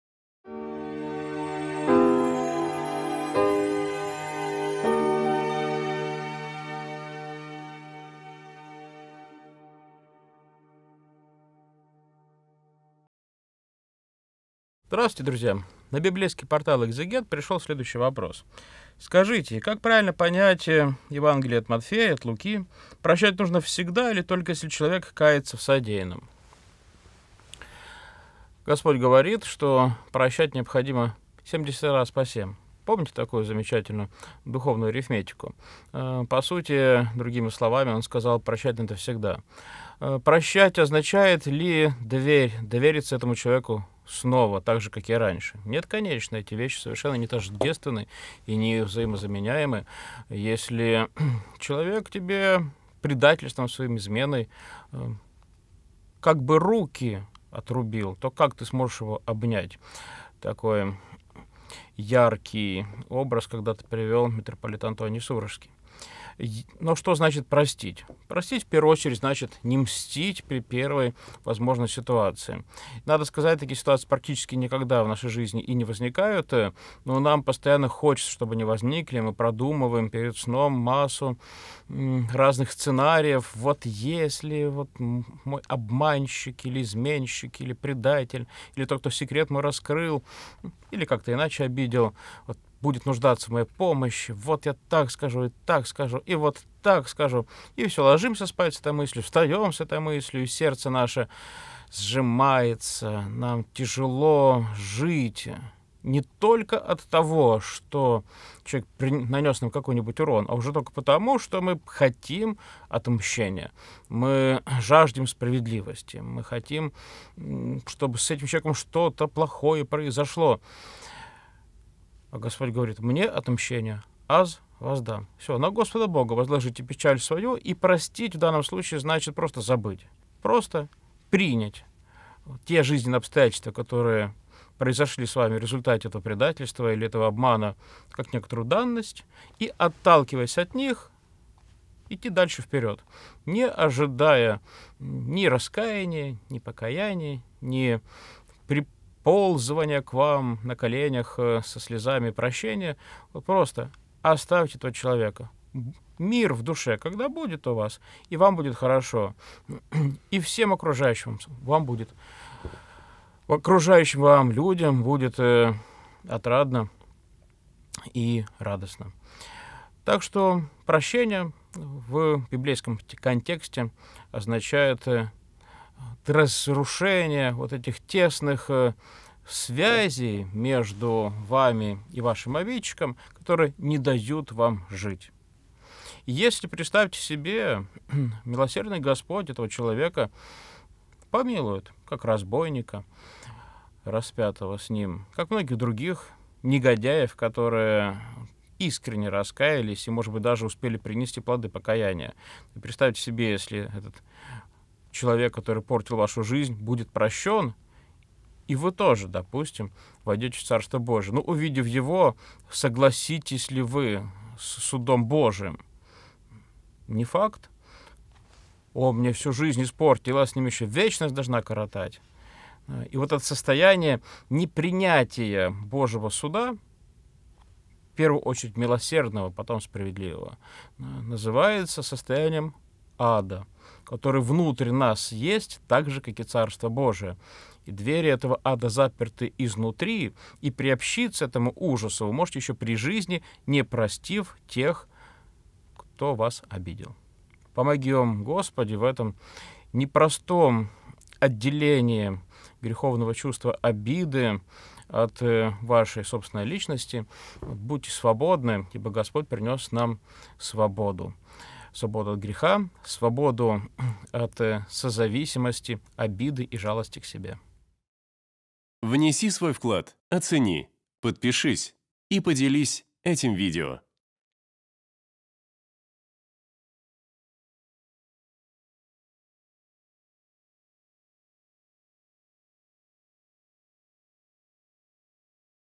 Прощать нужно всегда или только когда человек раскаивается ? Священник Владислав Береговой
Вместе со священником Владиславом Береговым отвечаем на вопрос: "Прощать нужно всегда или только когда человек раскаивается?".

🔸 Всегда прощать.
🔸 Потеря доверия.
🔸 Желание ответить обидчику.
🔸 Жажда справедливости.
🔸 Гнев Божий.
🔸 Полное прощение.

📜 Читать к Римлянам послание ап. Павла 12 глава 19 стих (с толкованиями)